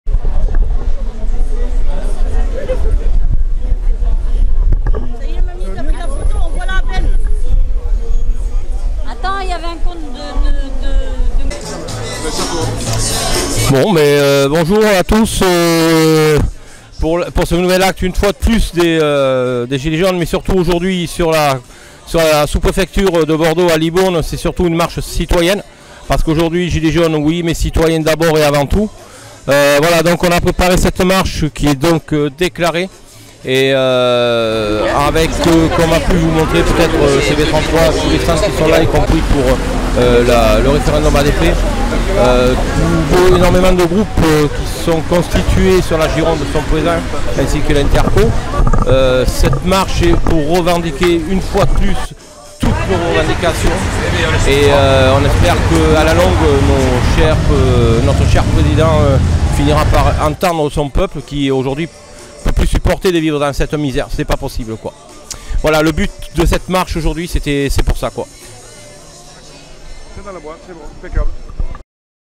interview d'un Gilets Jaunes Manif du samedi 14 septembre 2019
Mis en ligne le samedi 28 septembre 2019

Rassemblement pour la manifestation du 14 septembre 2019
un Gilets Jaunes nous expliquent le déroulement de la marche citoyennes avec plus de trois cents personnes dans les rues de LIBOURNE avec le village gilets jaunes,discussions, débats, prises, de paroles